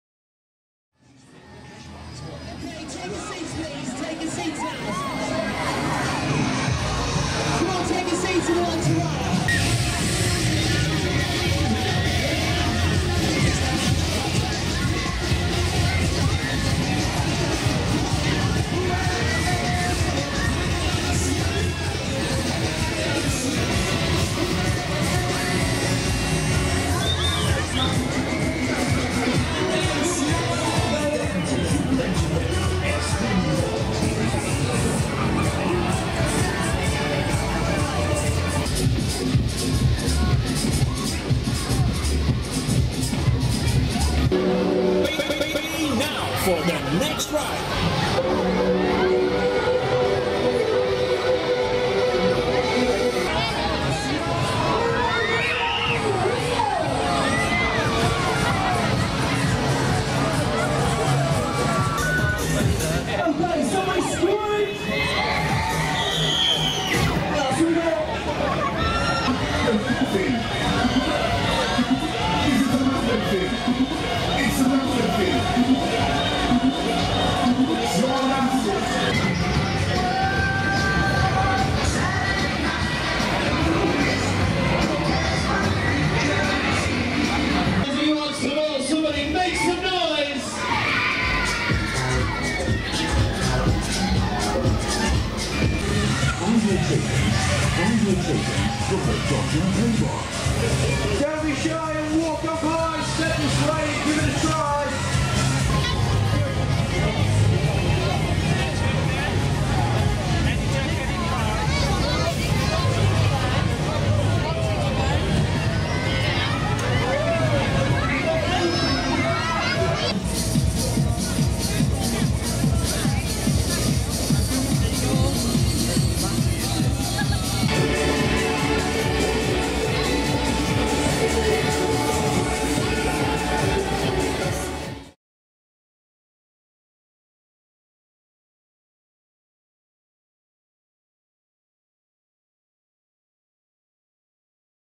Tewkesbury Mop Fair 2015 V2
Tewkesbury Mop Fair celebrates 800 years of Fairs (since 1199), it's the Biggest Street Fair in the West, with Traditional Rides and Stalls complemented with the very latest Thrill Rides, fun for all ages. Shops, Cafes, Pubs and Restaurants Stay Open while the Mop Fair takes over the Medieval Town centre with rides and stalls squeezed between fine Tudor Buildings and in some of the Town centre car parks.